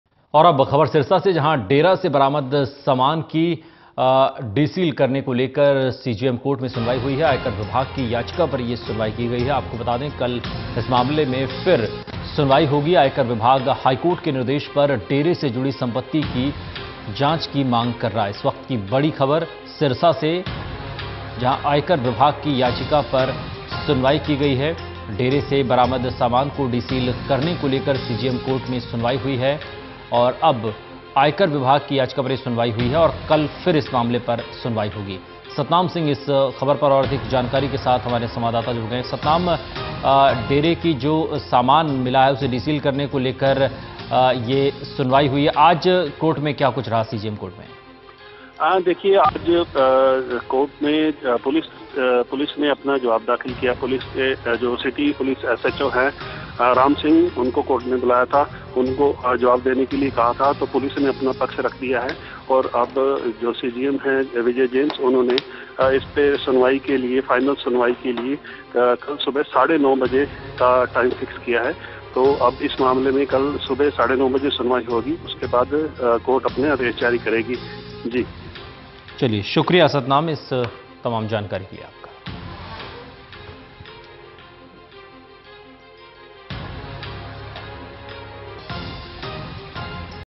सिरसा: डेरा सच्चा सौदा से बरामद सामान को सील करने के मुद्दे पर हुई सुनवाई
पुलिस द्वारा डेरा प्रकरण में बरामद समान को ड़ी सील करने की मांग को लेकर आयकर विभाग की याचिका पर आज  CJM कोर्ट में हुई सुनवाईं ।

कोर्ट ने सुनवाईं के लिए 31 अक्टूबर की तारीख मुकर्रर की ।

आयकर विभाग हाई कोर्ट के निर्देश पर  डेरा से जुड़ी सम्पति की जांच की कर रहा है मांग।